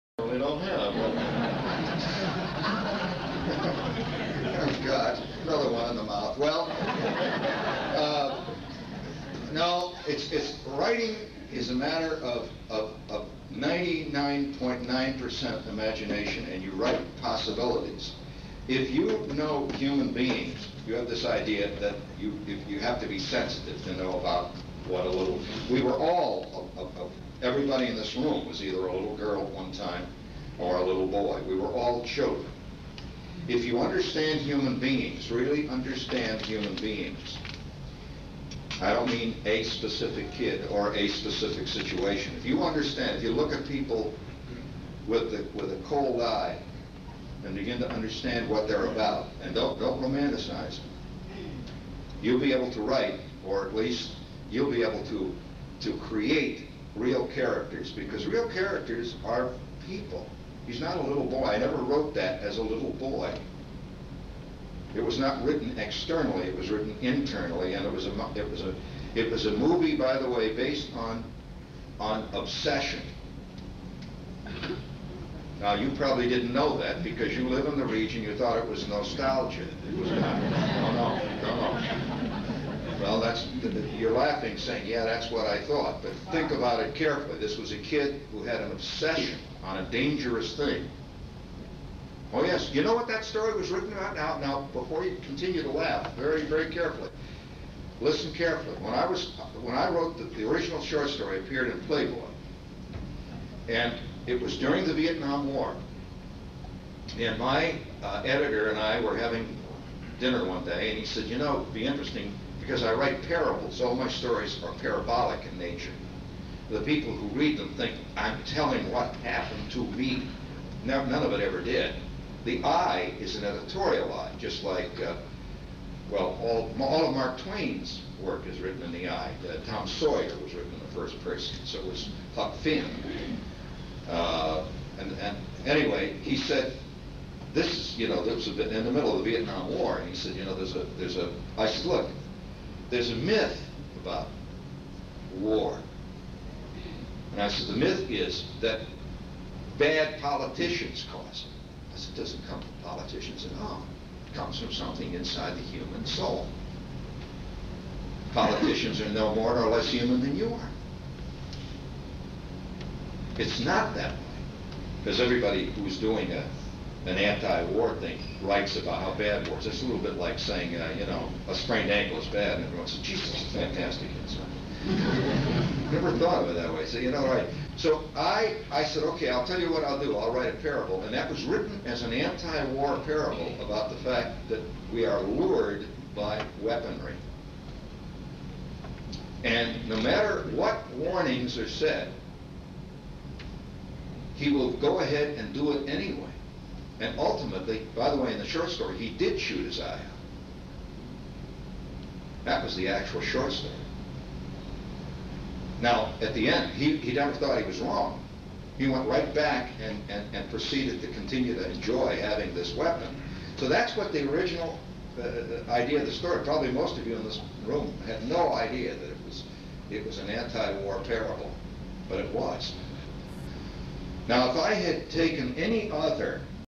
Jean Shepherd - Origin of BB Gun Story Lake County, Indiana 1984
In this RARE clip of Jean Shepherd, he visits the Lake County Library an discusses many facts about his work. In this clip, Jean offers the audience an insight as to where the Red Ryder BB Gun Story came from. I bet you didn't know!